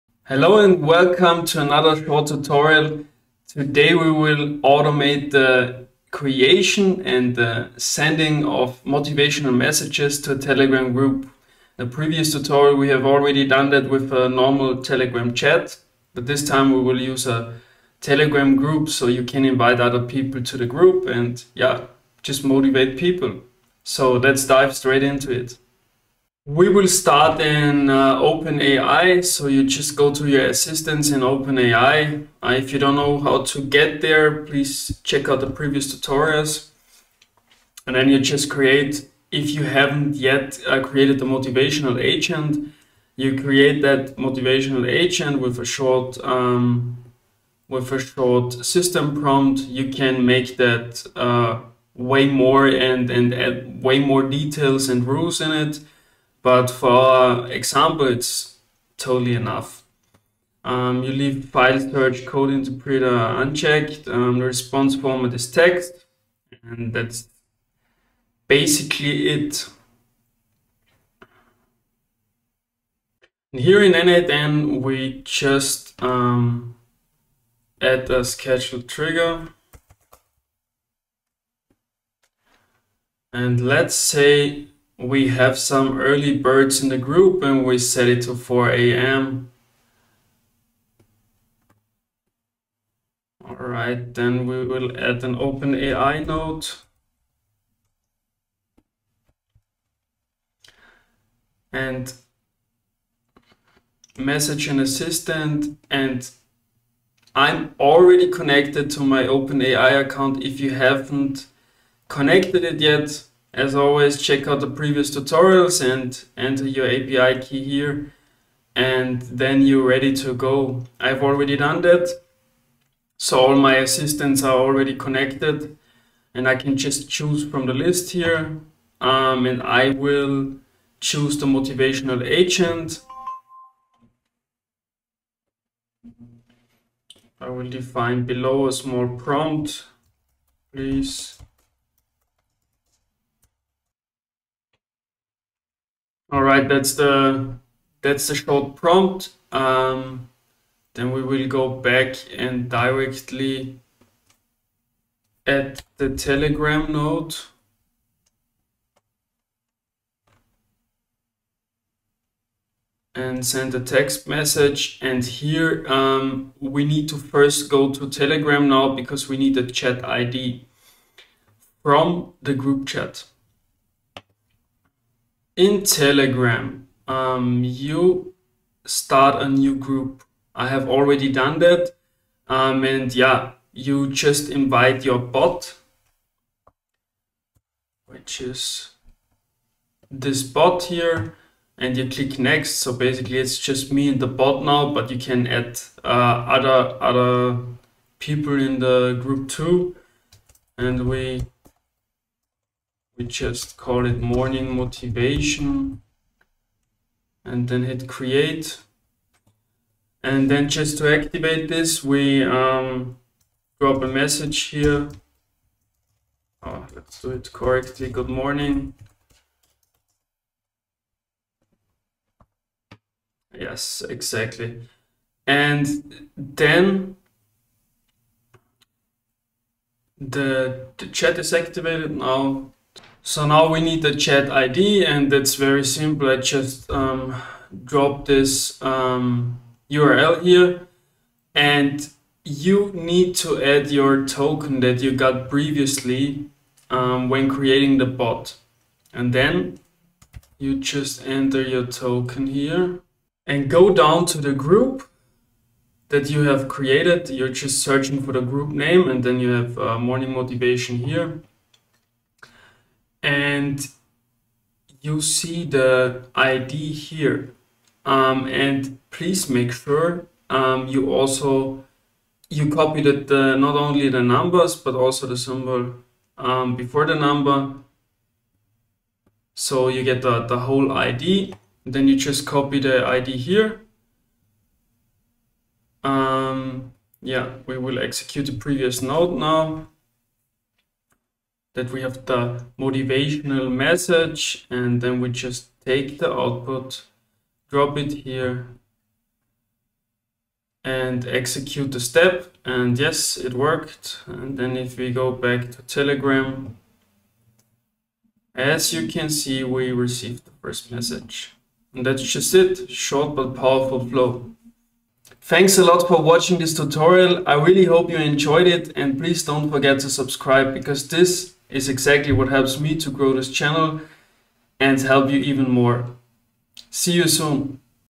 How to Send Messages to Telegram Groups with n8n (Motivation Bot Guide)
Yes — your Telegram bot can send messages to group chats! In this quick tutorial, I’ll show you how to set it up in n8n.

✅ What we’ll cover:
- How to invite your Telegram bot to a group
- How to send a message to the group
- How to use getUpdates to find the correct `chat_id`
- How to send smart messages to your Telegram group using n8n

📦 Use Case:
We respond to a viewer comment on YouTube and show how to use n8n to send motivation messages to a Telegram **group**, not just private chats.

🛠 Tools used:
- n8n (Cloud)
- Telegram Bot
- OpenAI (optional, if you want to auto-generate messages)

💡 Tip: Don’t forget to promote your bot to **admin** in the group for it to send messages!